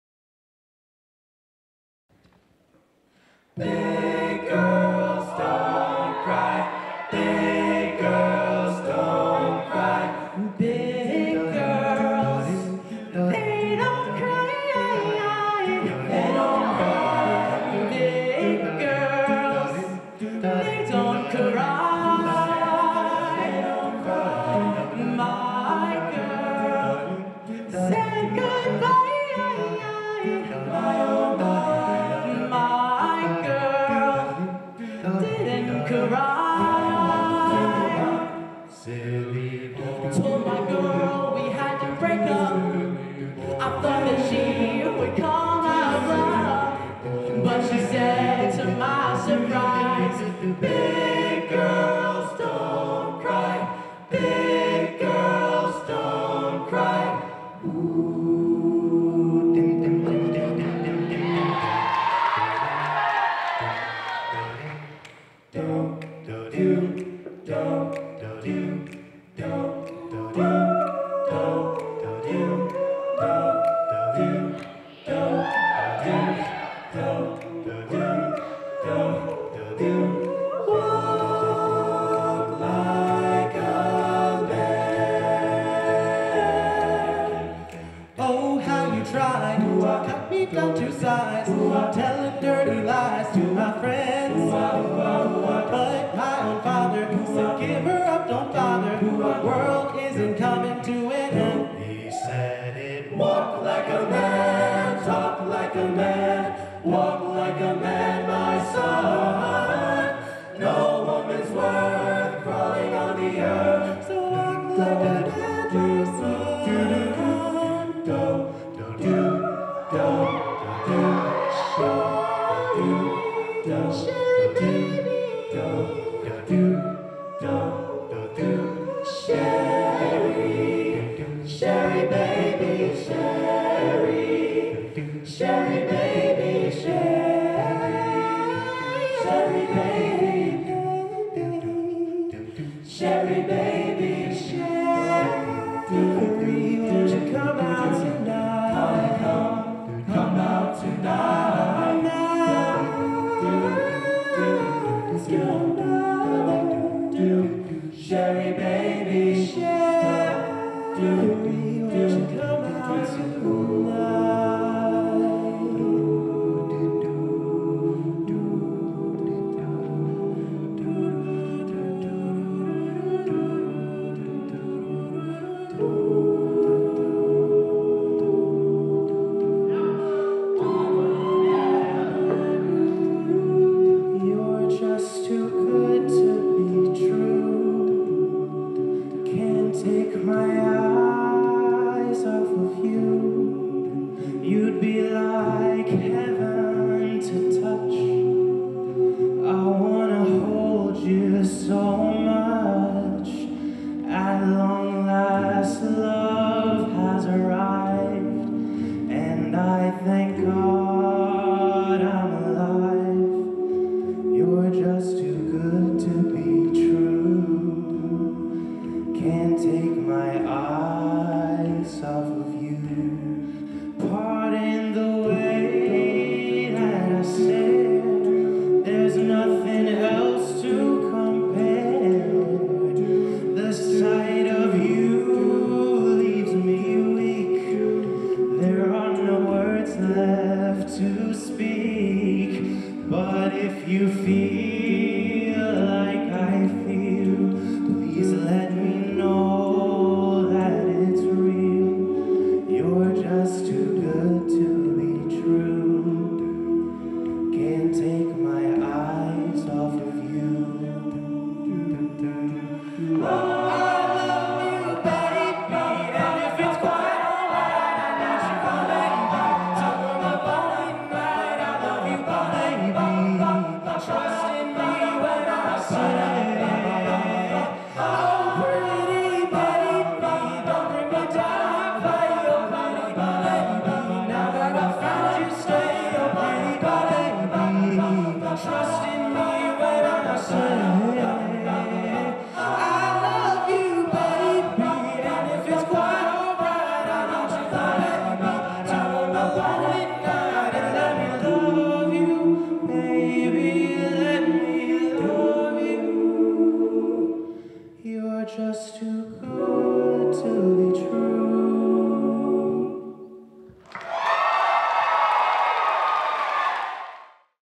Frankie Valli Medley | The Virginia Gentlemen
The Virginia Gentlemen perform an original medley of songs from Frankie Valli and The Four Seasons at the 2022 Fall Concert.

Soloist: Josh Bertrand, Cavan Meade, Patrick Cloud, Philip DiMeglio
Arrangement: Yo Han Hong